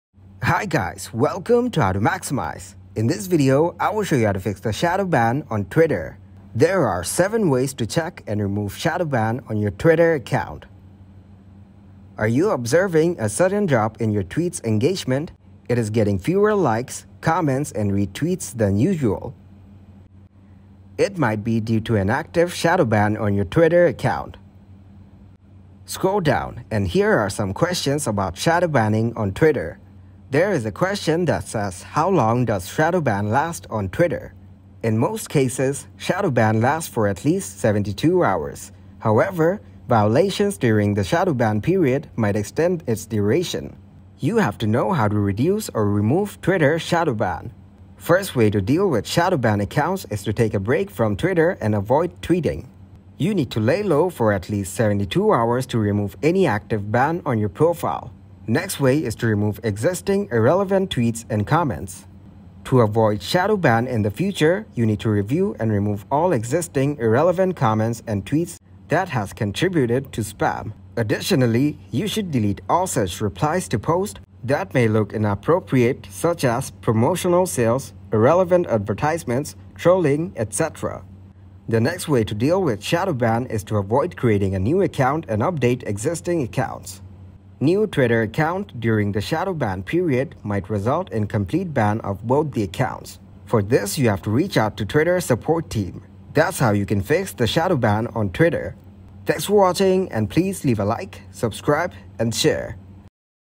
How to Fix Shadowban On Twitter (Easy 2023)
In this tutorial, you will learn How to Fix Shadowban On Twitter in easy steps by following this super helpful tutorial to get a solution to your problem!!